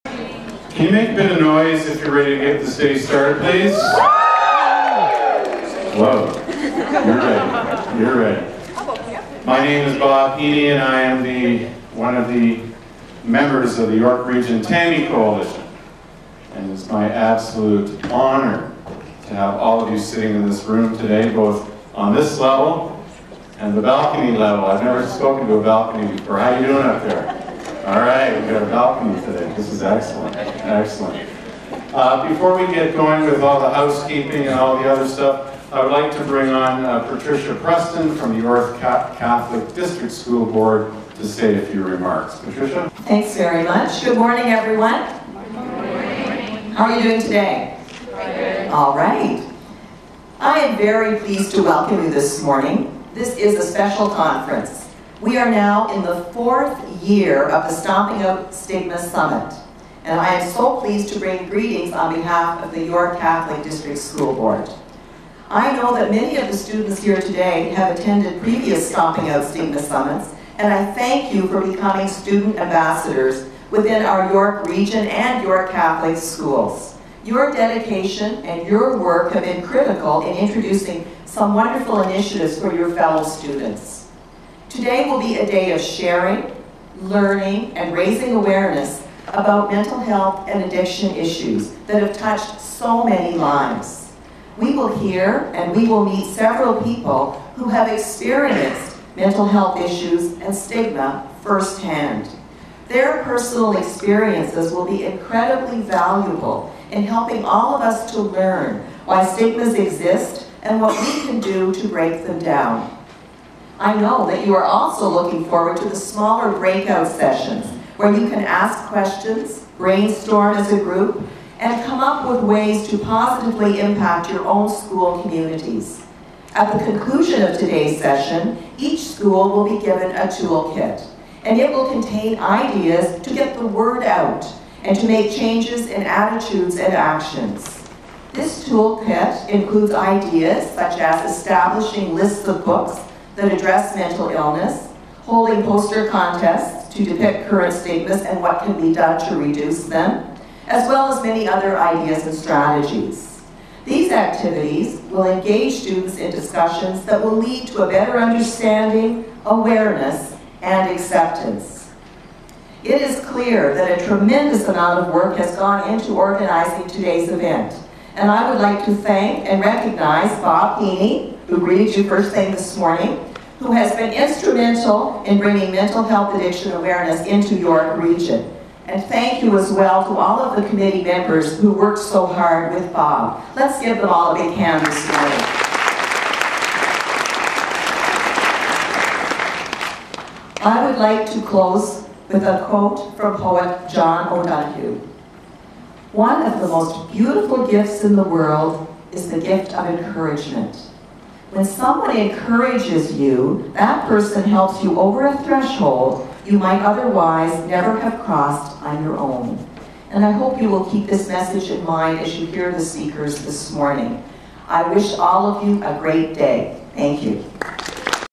Bob Heeney & Patricia Preston Open the 2010 Stomp Out Stigma Conference
Bob Heeney (Ontario Shores Centre for Mental Health Sciences) and Patricia Preston (York Region Catholic District School Board) welcomes the full house of youth and teachers to the 4th annual Stomp Out Stigma Conference.